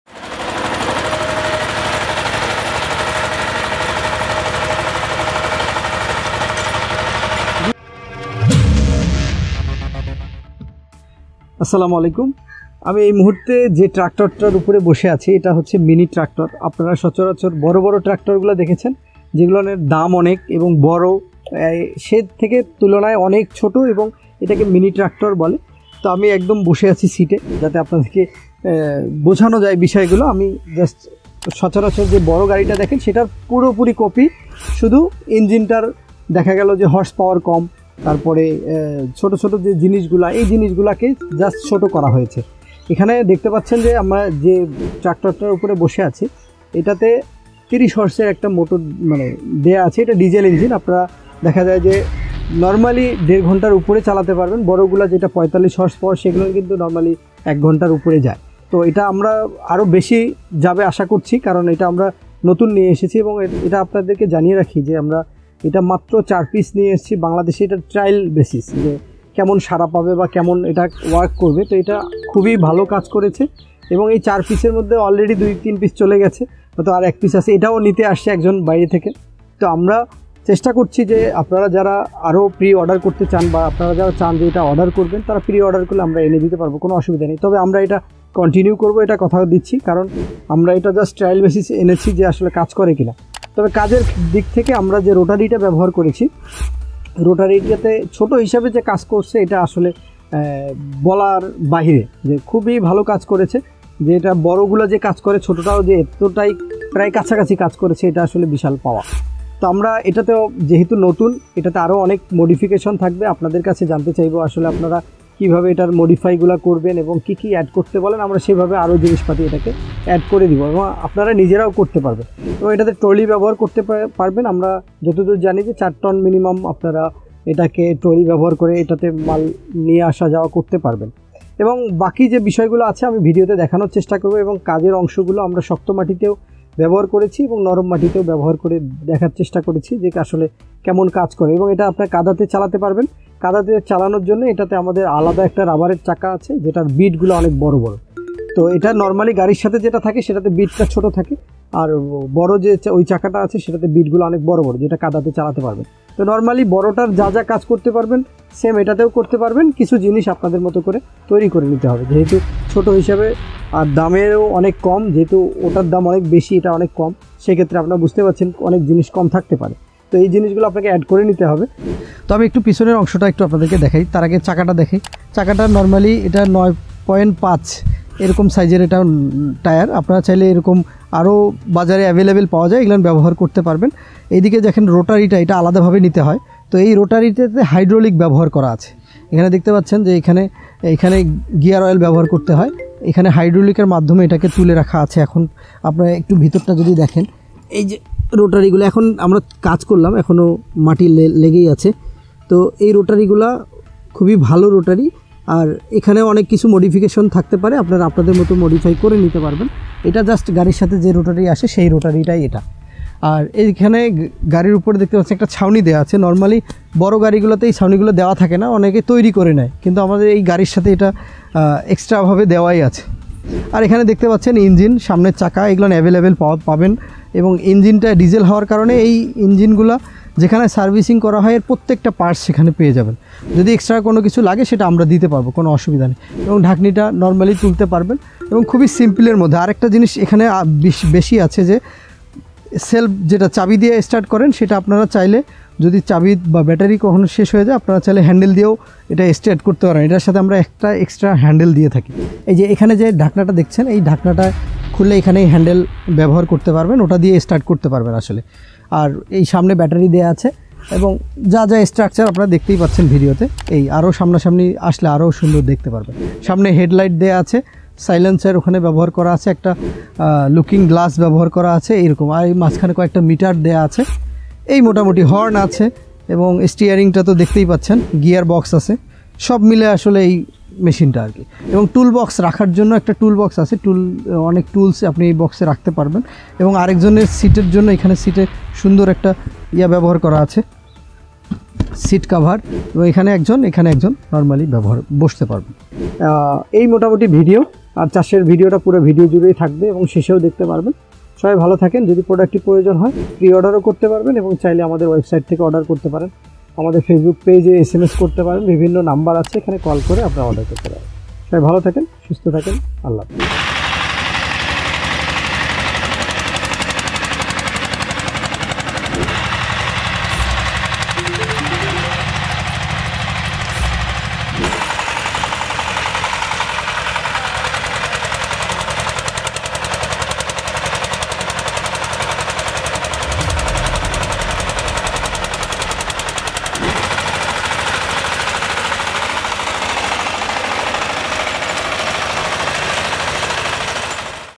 দেশের প্রথম মিনি ট্রাক্টর পাচ্ছেন সুলভ মূল্যে।Mini Tractor 30 Hp। Krishi Bazar
মিনি টিলার ৩০ হর্স পাওয়ার
মূল্য : ৩,৯০,০০০ টাকা।